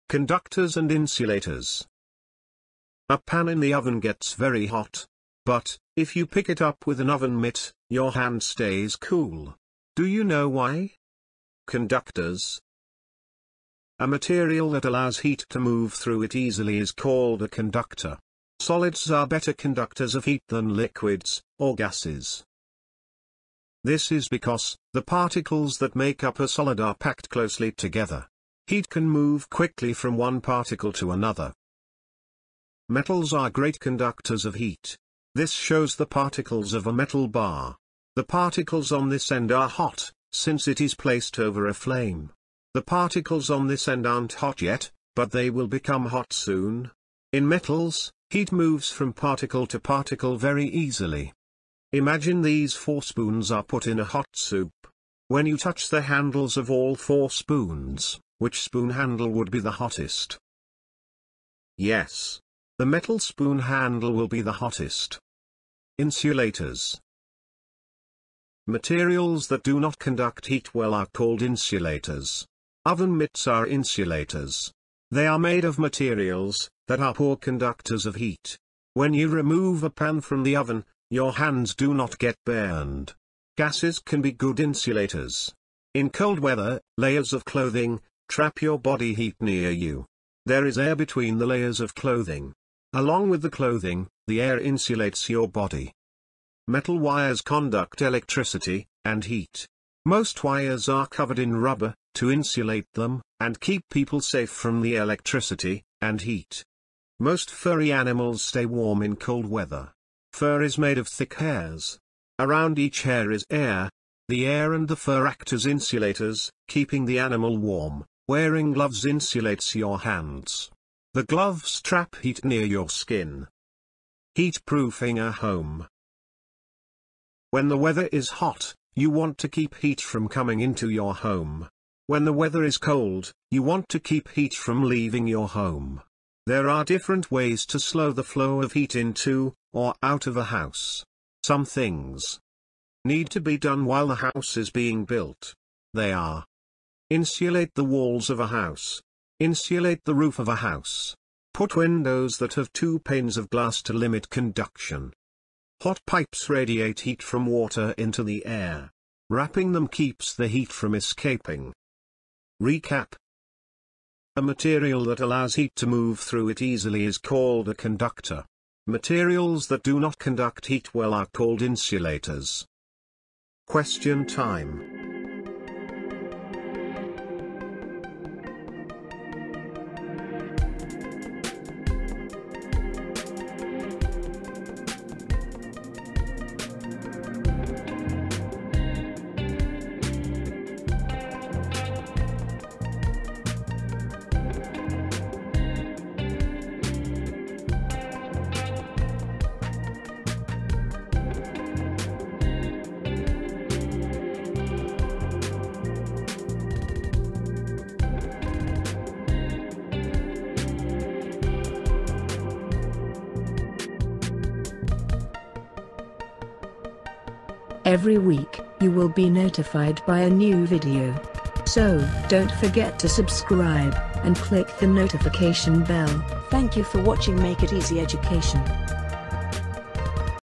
CONDUCTORS AND INSULATORS || SCIENCE EDUCATIONAL VIDEO FOR KIDS
THIS VIDEO EXPLAINS ABOUT CONDUCTORS AND INSULATORS. IT IS USEFUL FOR KIDS AND CHILDREN.